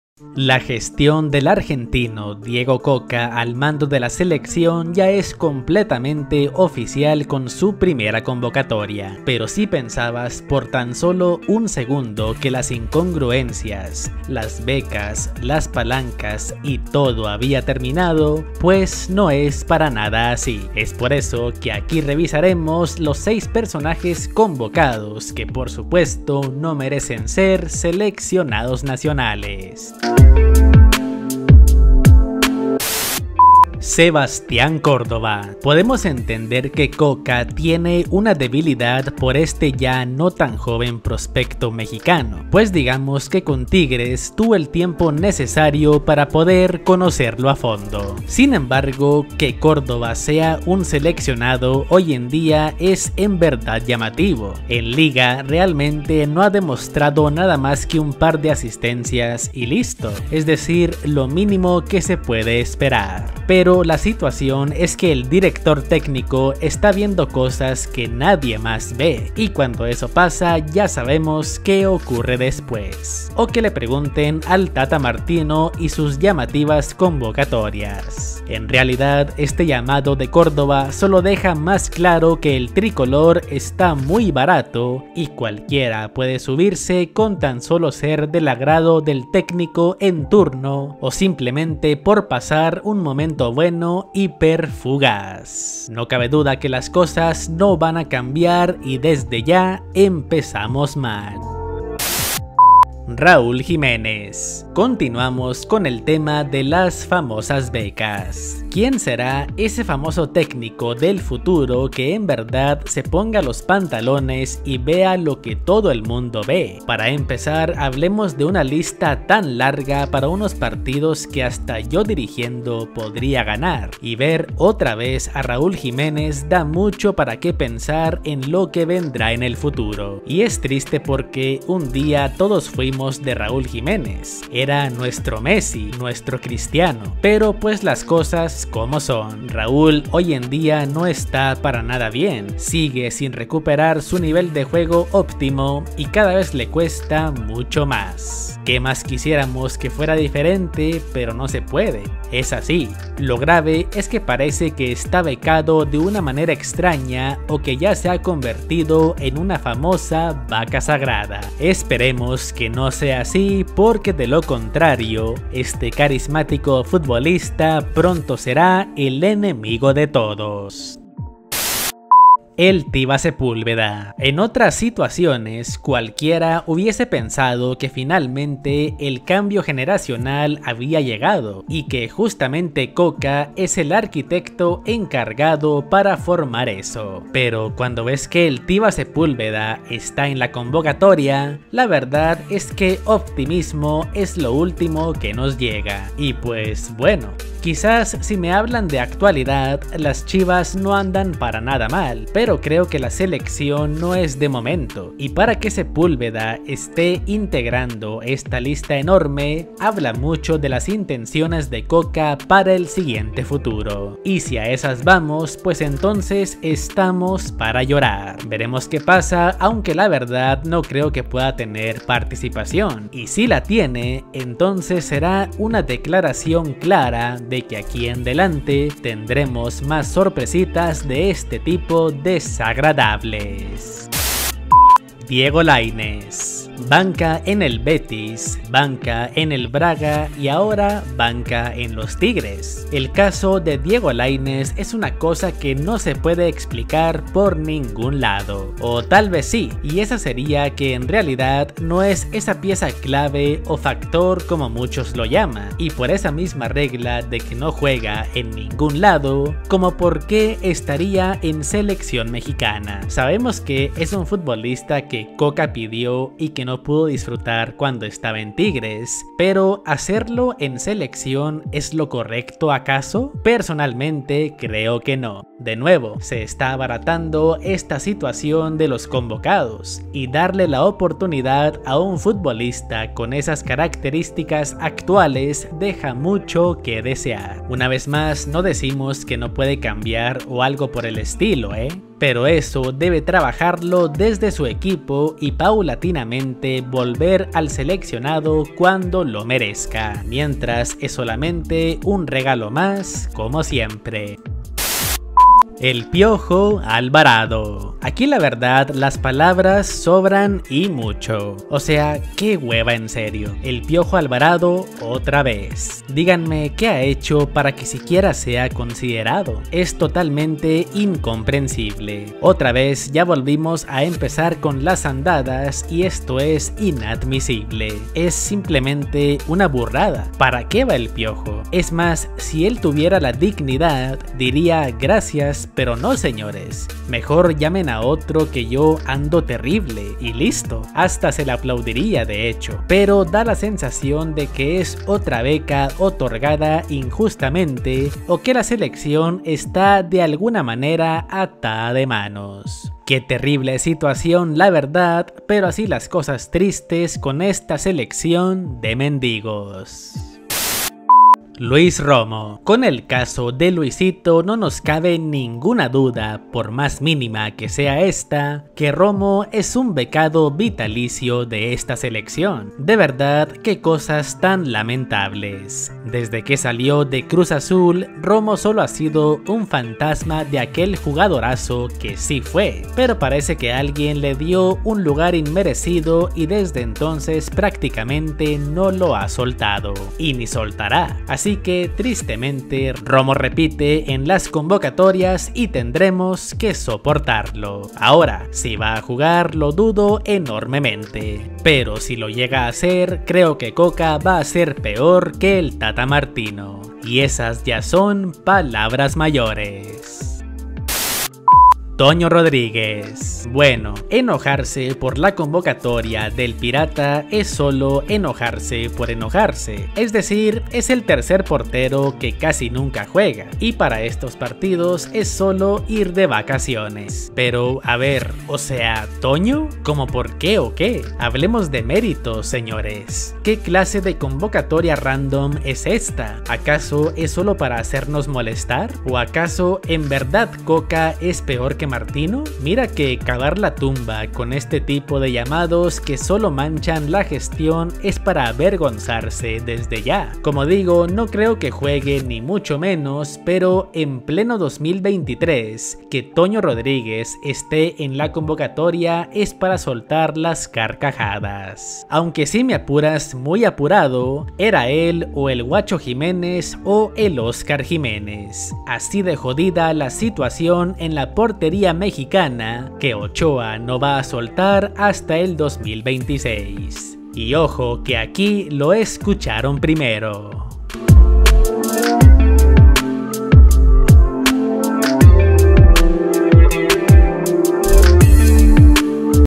Los 7 futbolistas que no deberían estar en Selección, Primer llamado de Cocca y nada cambia
La gestión del Argentino Diego Cocca al Mando de la selección ya es completamente oficial con su primera convocatoria, pero si pensabas por tan solo 1 segundo que las incongruencias, las becas, las palancas y todo había terminado, pues no es para nada así, es por eso que aquí revisaremos a 6 personajes convocados que por supuesto, no merecen ser seleccionados.